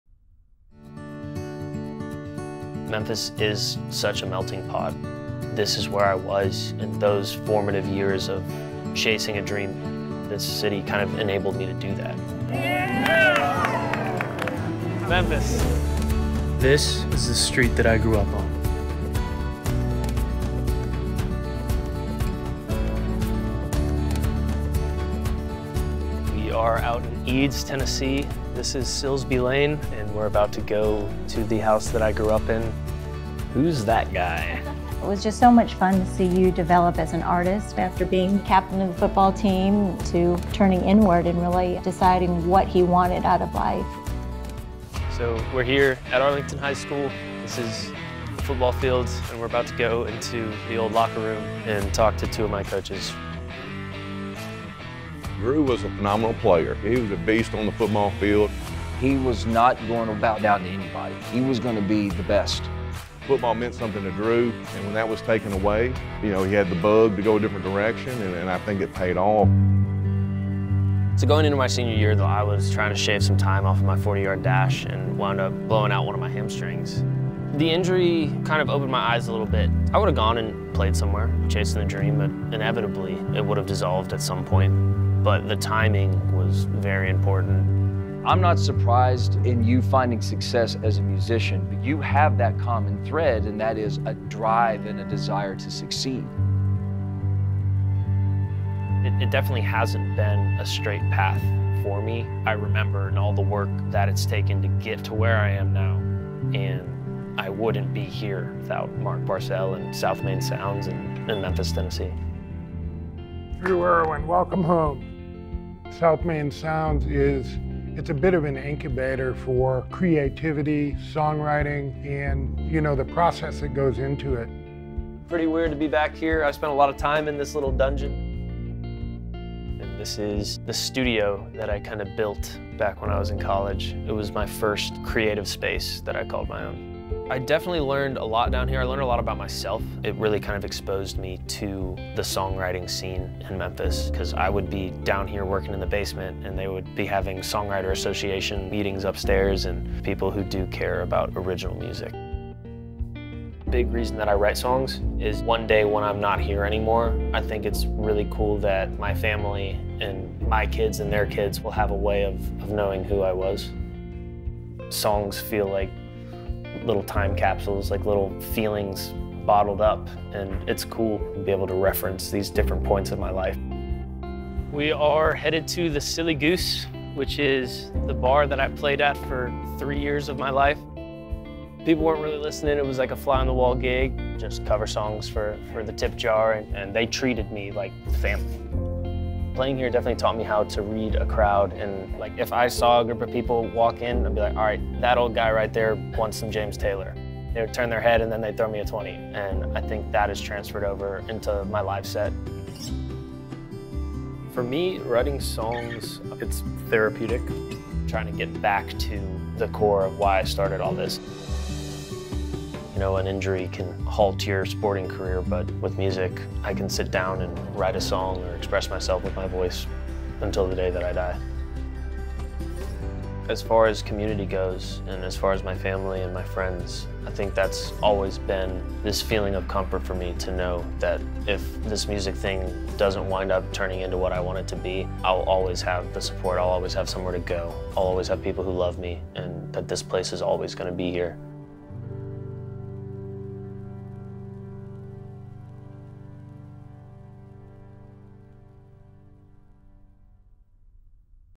State Farm® + UPROXX Present: Street I Grew Up On With Drew Erwin
Drew Erwin goes back to his hometown to detail how the melting pot of Memphis helped shape his life and moving on from football to pursue his second passion of writing music and becoming a recording artist in the Street I Grew Up On with @State Farm